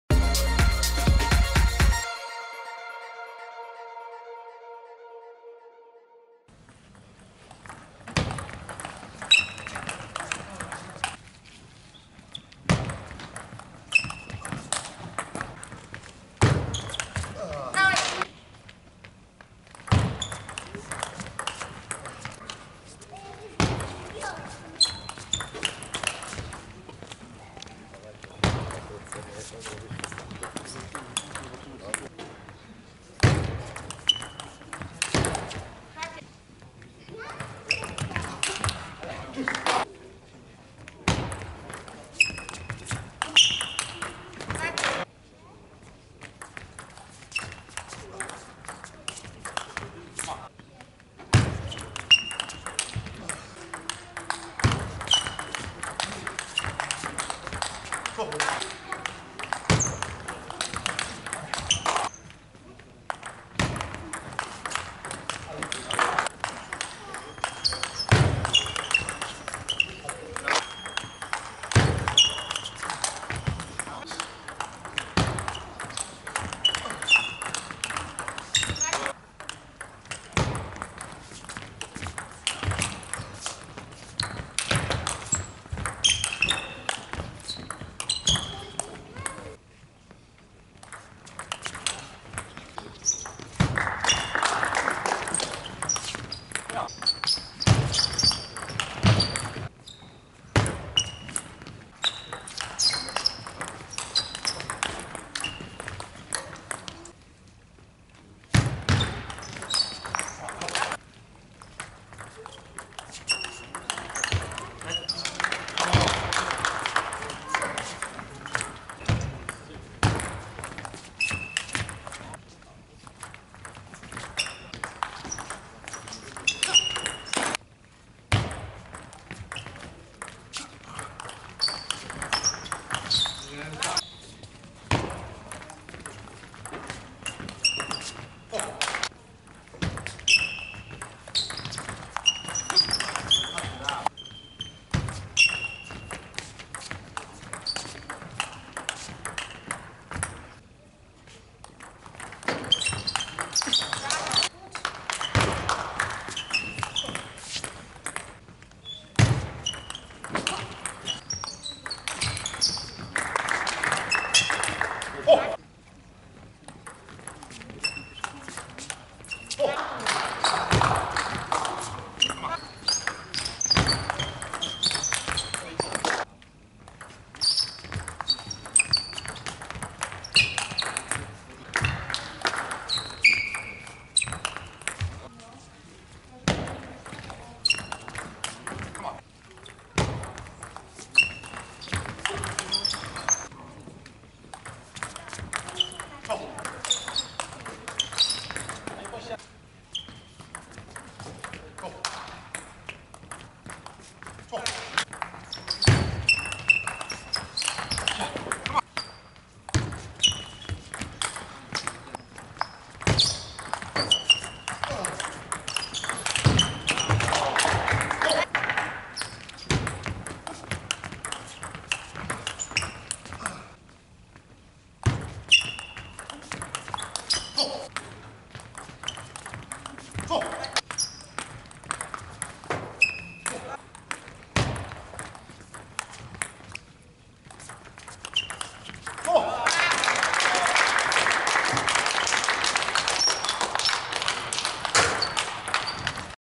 Marek Kądziela - Jakub Załuski
Mecz w ramach spotkania 2 ligi śląsko - opolskiej
KTS Gliwice - BLYSS Kujakowice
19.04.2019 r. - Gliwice
Grają: